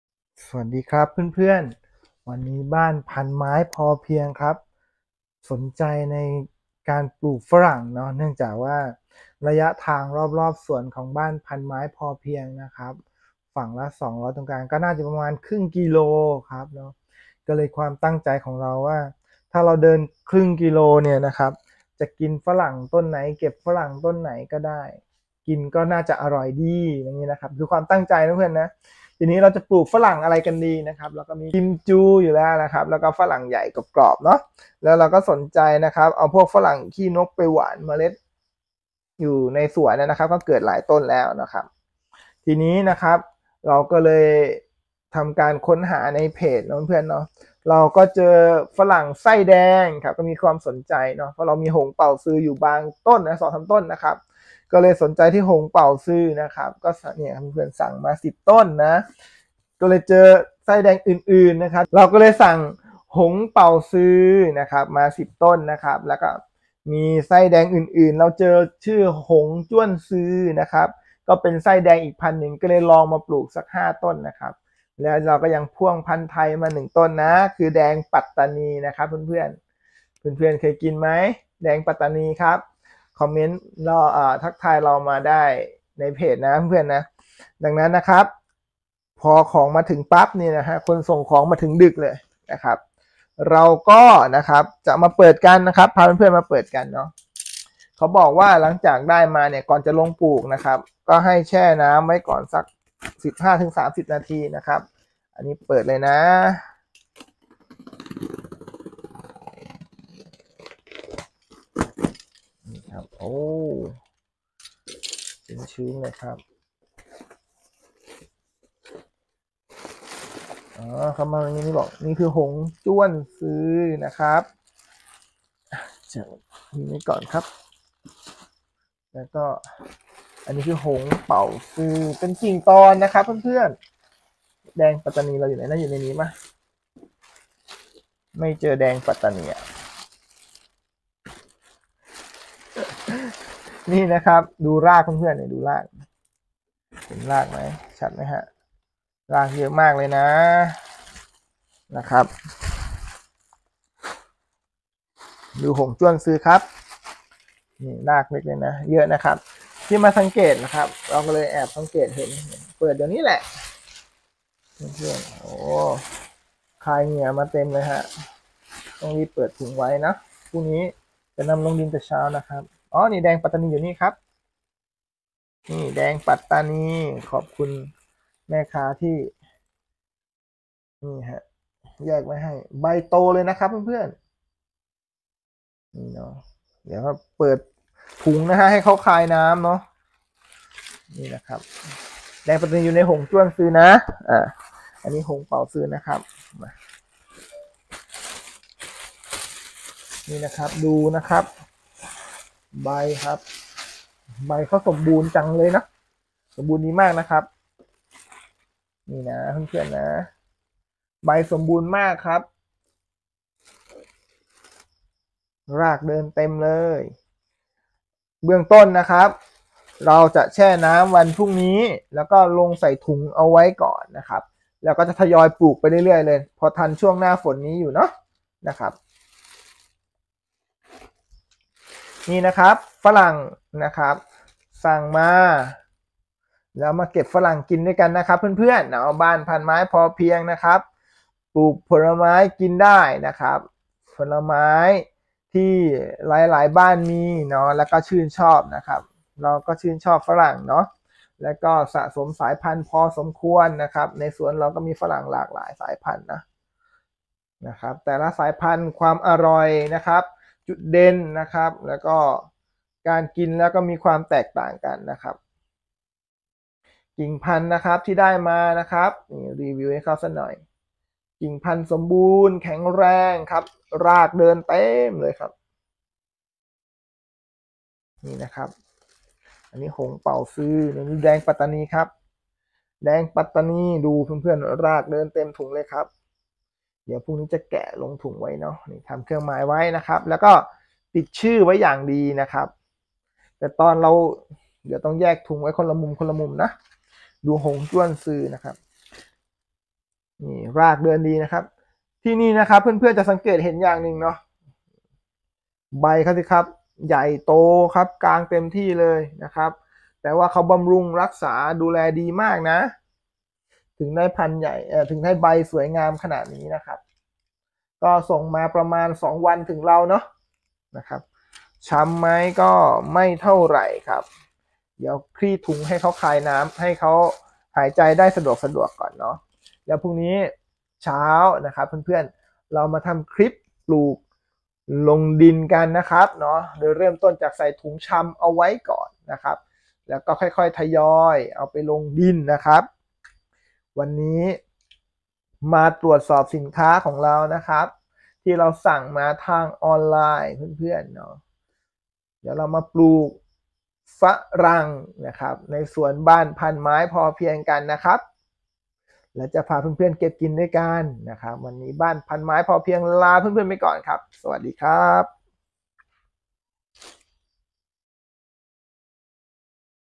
ปลูกฝรั่งไส้แดง วันนี้พามาชมกิ่งตอนฝรั่งไส้แดง
ปลูกฝรั่งไส้แดง วันนี้พามาชมกิ่งตอนฝรั่งไส้แดง หงส์เป่าสือ หงส์จ้วนซือ แดงปัตตานี จะนำมาปลูกไว้ทานที่คันนาตลาดสดของเรากันจ้า